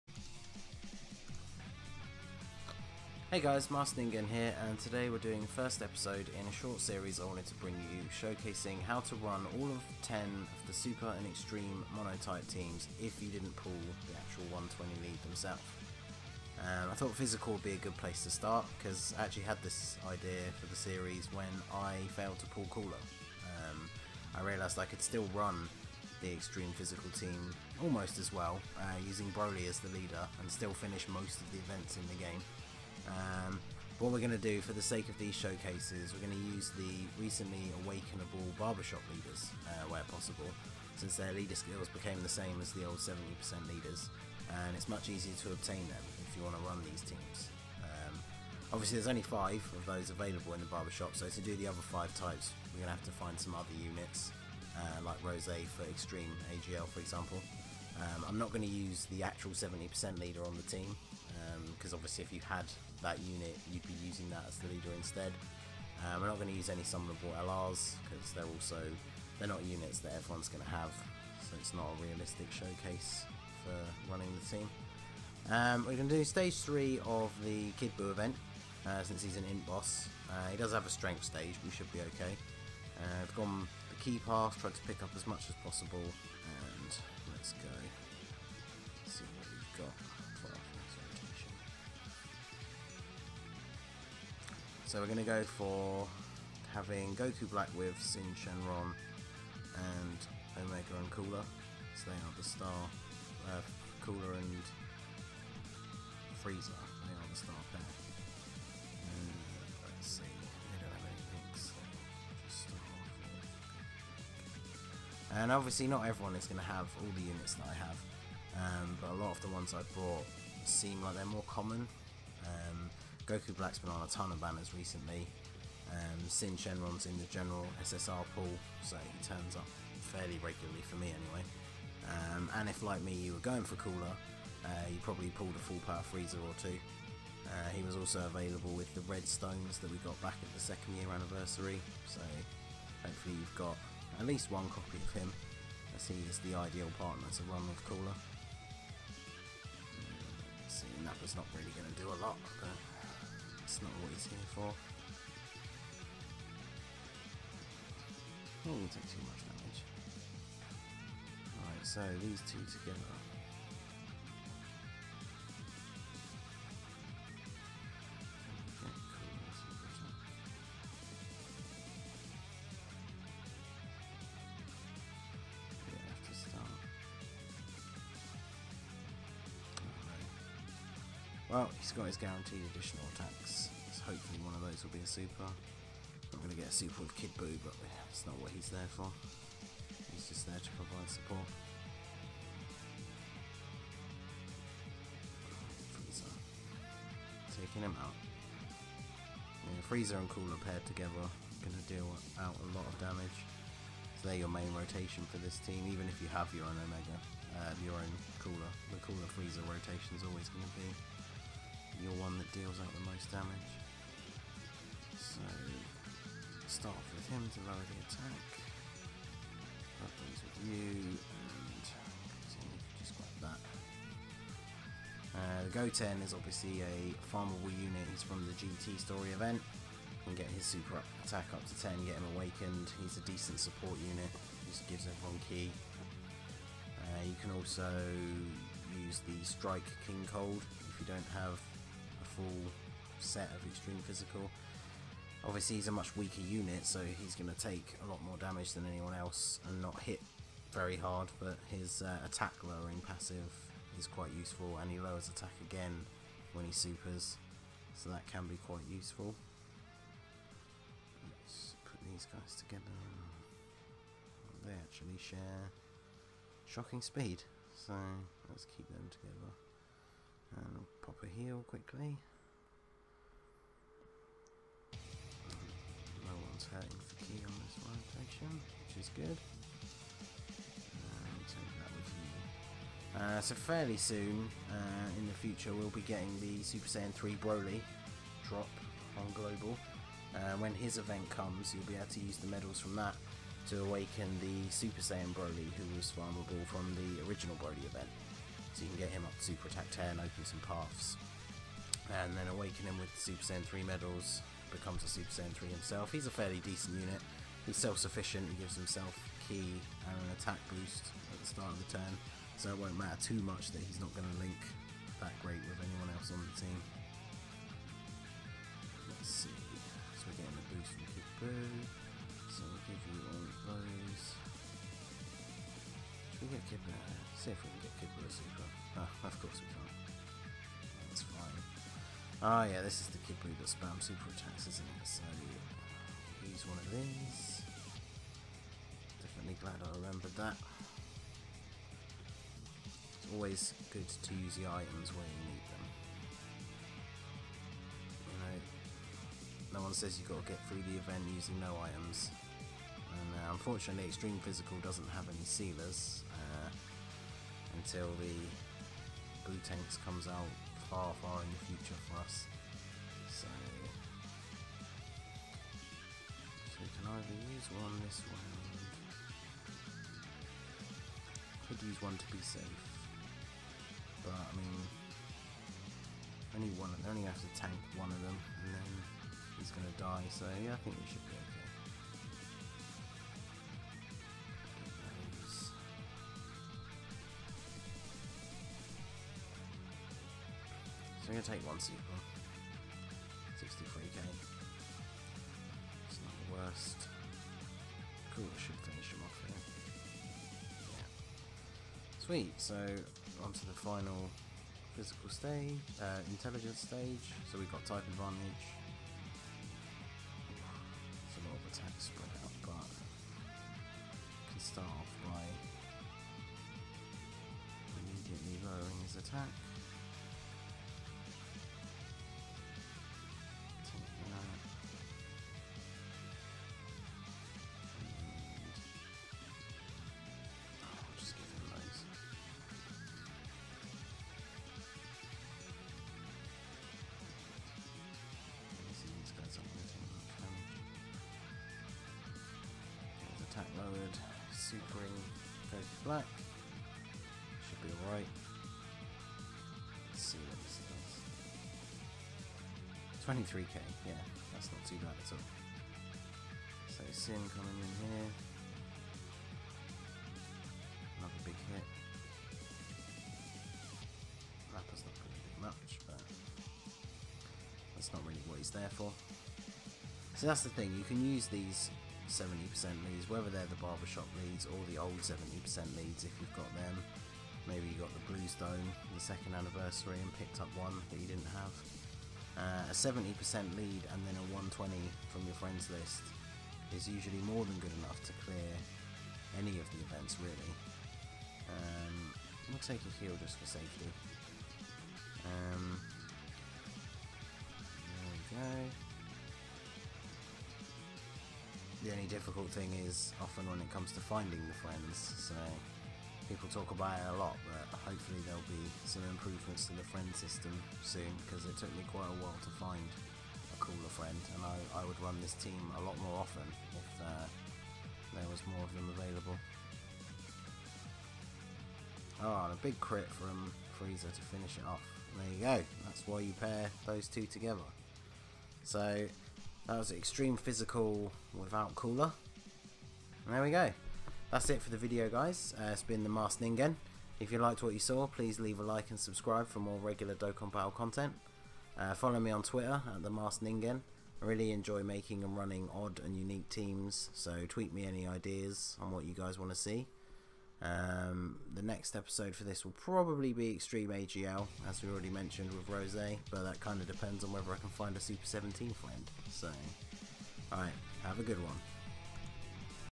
How to Run Series: Extreme PHY without Cooler!
In this first episode of the How to Run series, I'll be showcasing an Extreme Physical team using the Baba shop Nappa as the leader instead of Final Form Cooler. Does the team have what it takes?